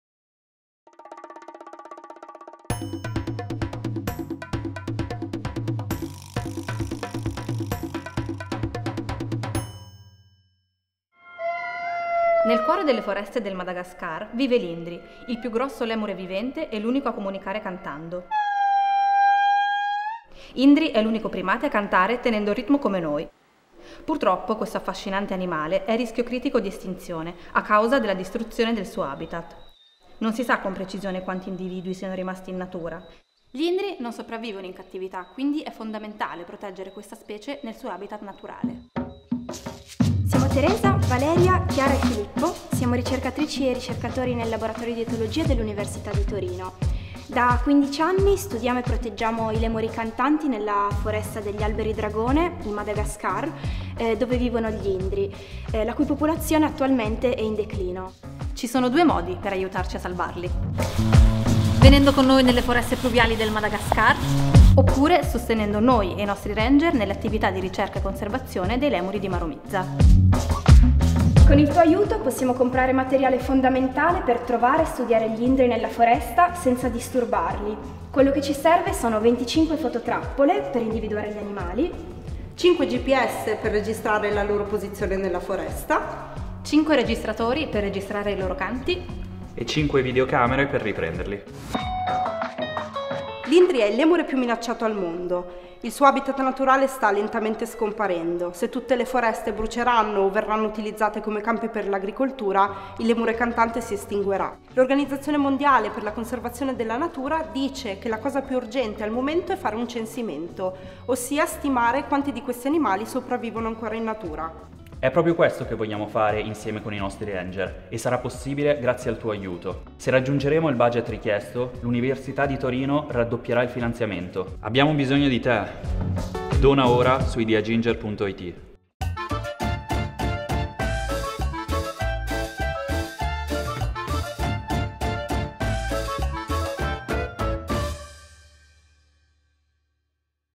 Salviamo il lemure cantante del Madagascar
Indri, unica specie di lemure cantante, è a rischio di estinzione. Aiutaci a salvarlo! Dona su ideaginger.it

L’indri (Indri indri) è l’unico tra i lemuri a comunicare attraverso un vero e proprio canto, che riecheggia nelle foreste del Madagascar. È a rischio critico di estinzione e non sopravvive nei parchi zoologici, quindi va assolutamente protetto nel suo habitat naturale.
Noi etologi e primatologi dell’Università di Torino abbiamo bisogno di te per studiare, monitorare e salvaguardare la popolazione di indri in natura, per evitare che si estingua. Aiutaci a formare ed equipaggiare i nostri ranger per scovare, conoscere e monitorare i gruppi di indri nella foresta pluviale di Maromizaha.

Se grazie alla tua donazione raggiungeremo il budget richiesto, l’Università di Torino raddoppierà il finanziamento. UniTo lancia la sfida: tu che fai, lasci o raddoppi?